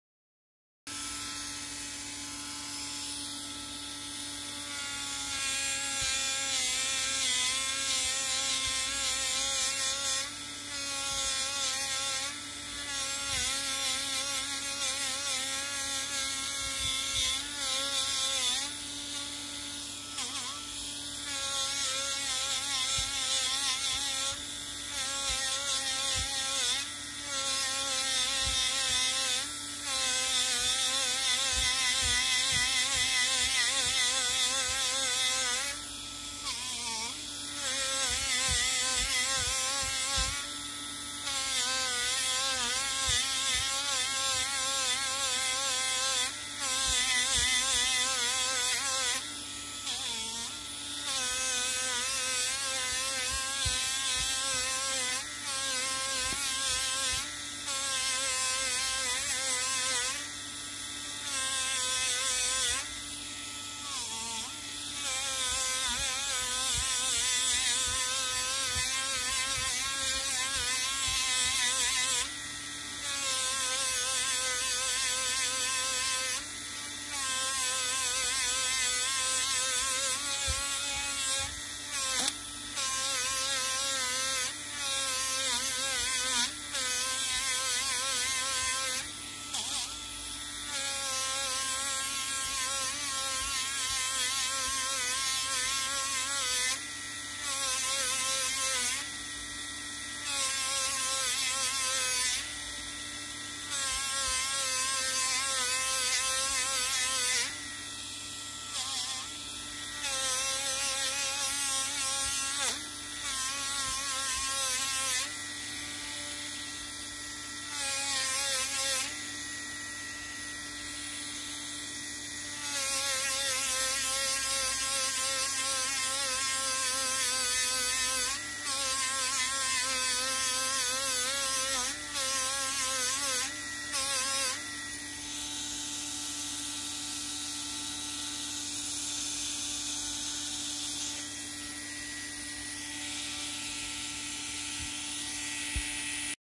朱銅 亜酸化銅 アトリエ原澤
銅を赤く光る位熱して冷まします。その後銅の黒い被膜を磨くと亜酸化銅の赤が出てきます。